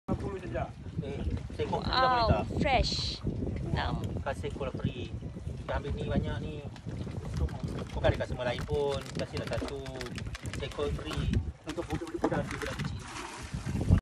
Live Flower Crab in Mabul Island
This is video was recorded in Mabul Island, Sabah, Malaysia in early December 2021. As you can see the seller is on a kayak selling live and fresh flower crabs that is to be cooked in any preferred restaurant in Mabul Island. The price is easily negotiable because there is not much visitors during this time. As a summary, please enjoy this short video of me with crabs and the beautiful clear/turquoise sea :)